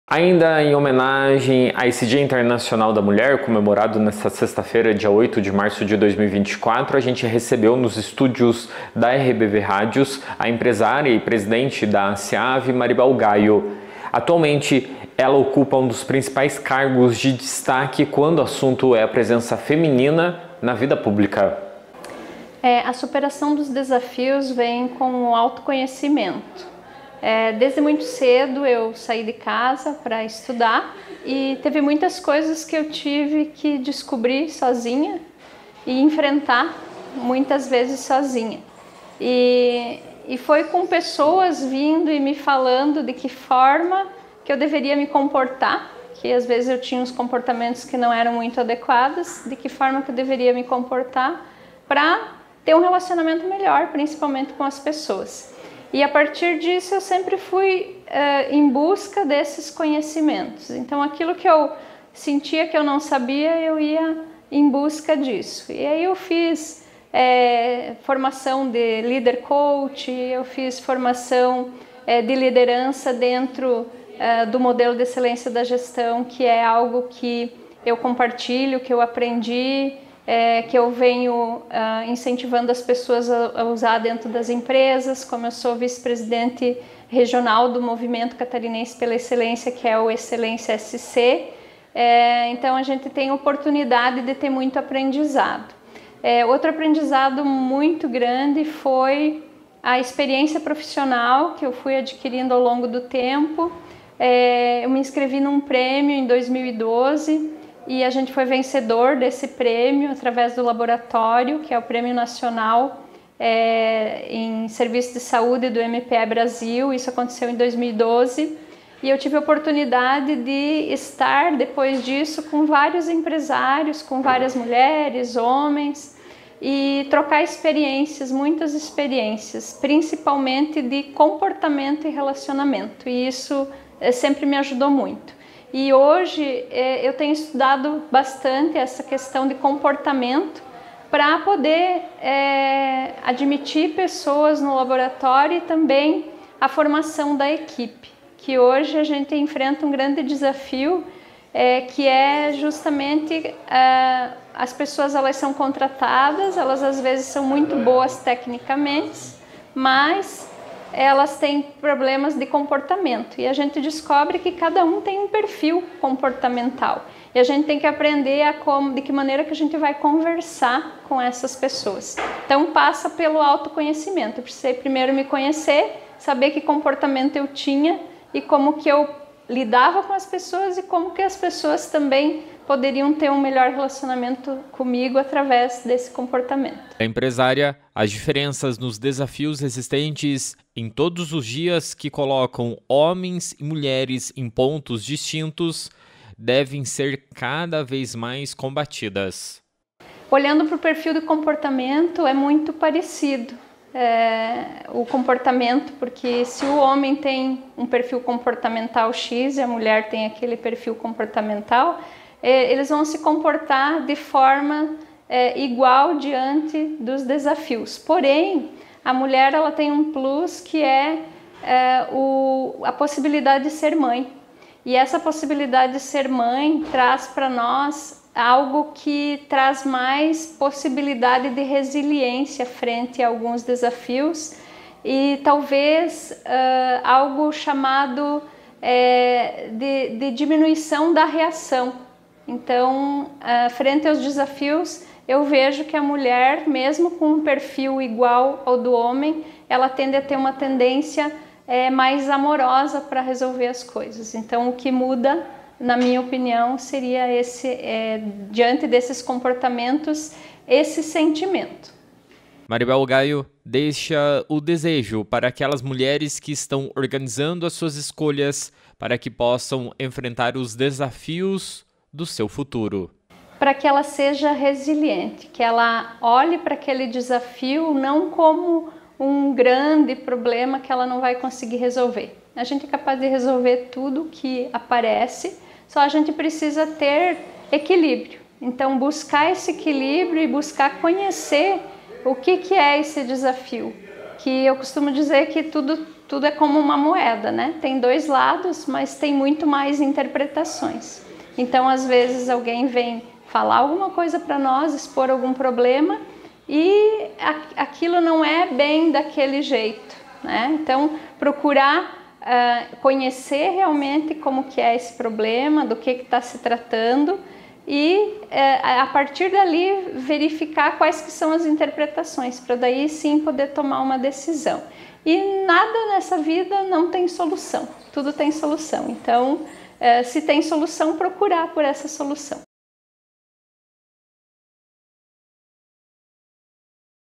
Os desafios da mulher no mundo corporativo
A mulher enfrenta inúmeros desafios na sociedade, e ser líder em uma empresa ou na liderança de um grupo tem muitos desafios. Ernesto Júnior traz uma reportagem com a presidente da Associação Empresarial de Videira, Maribel Gaio.